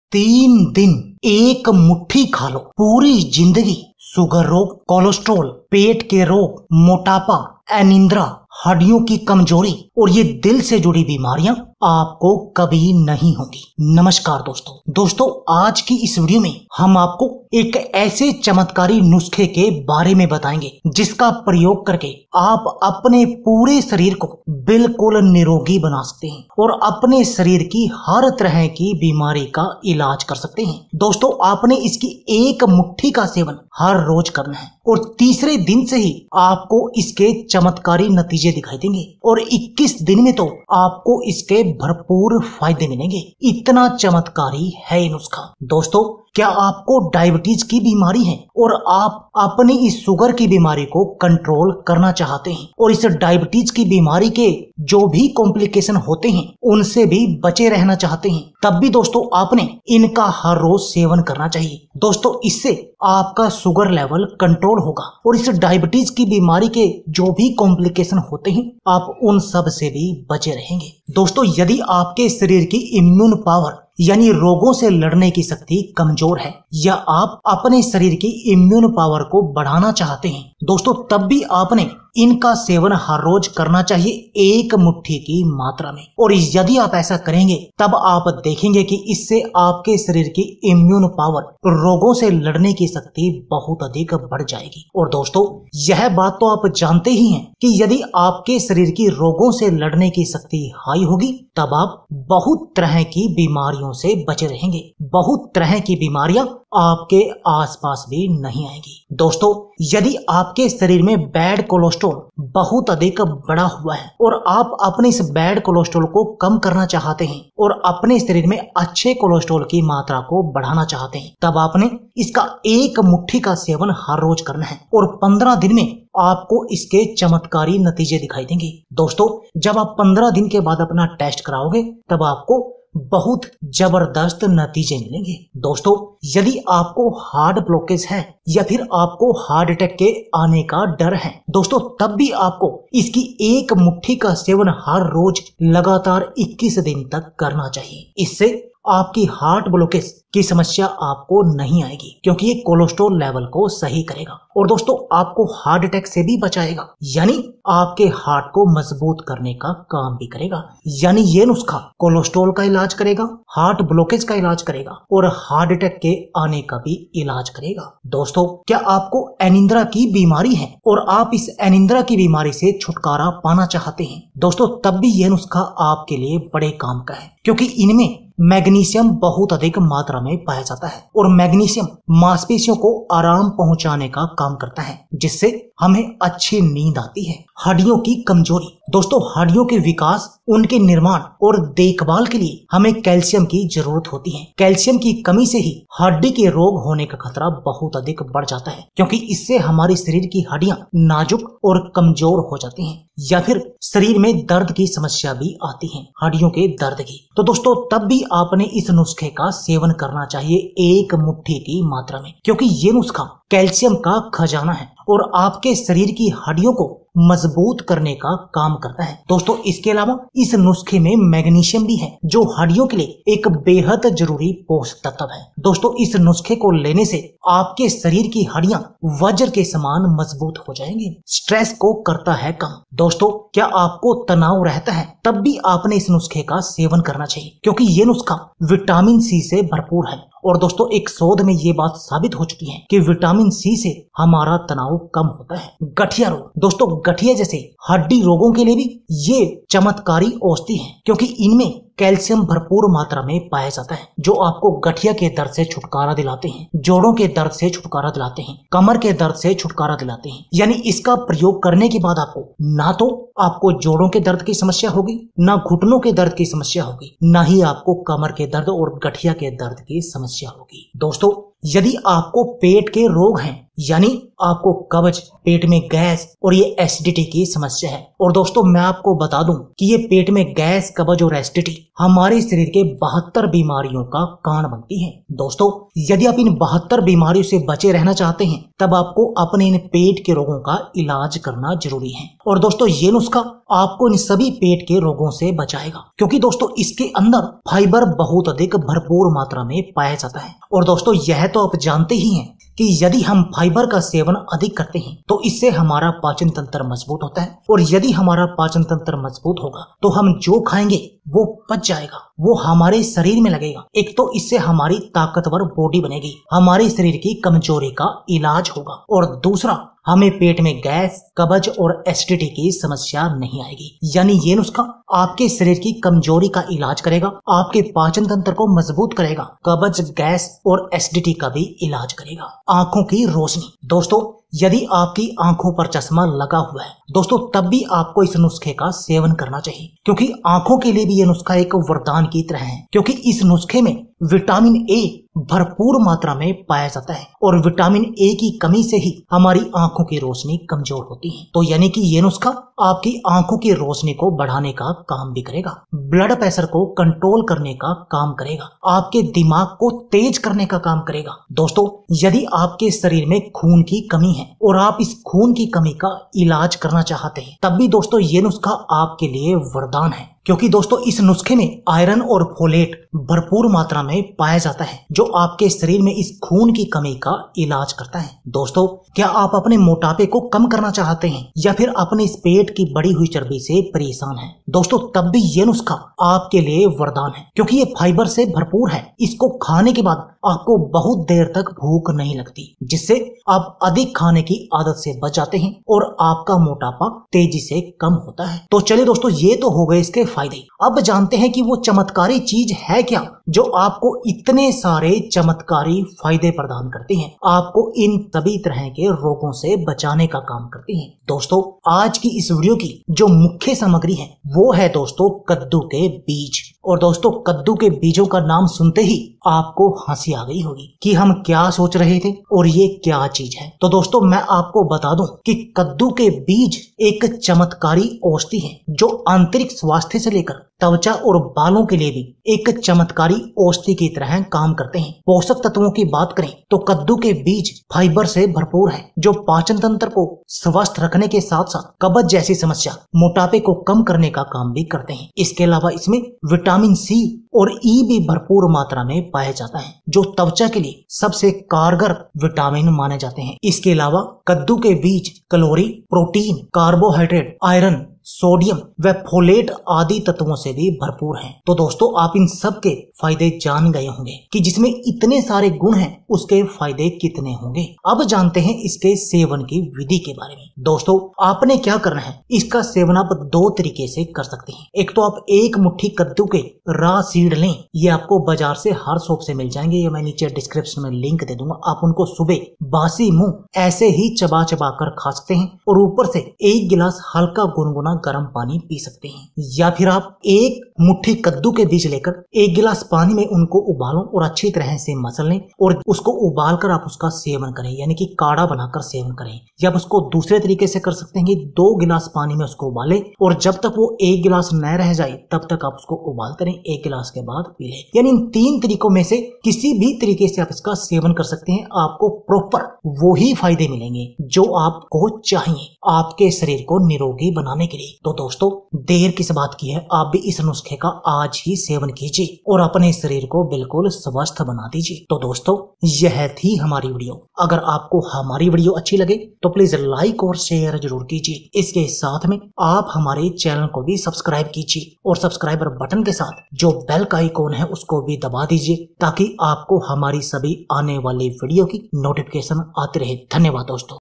3 दिन खालो पूरी जिंदगी शुगर कोलेस्ट्रॉल पेट रोग मोटापा अनिद्रा हड्डियो और दिल की बीमारियाँ नही होंगी
3 दिन सिर्फ 1 मुट्ठी खा लो, पूरी जिंदगी शुगर रोग, कोलेस्ट्रॉल, पेट के रोग, मोटापा, अनिद्रा, हड्डियों की कमजोरी और दिल की बीमारियाँ कभी नहीं होंगी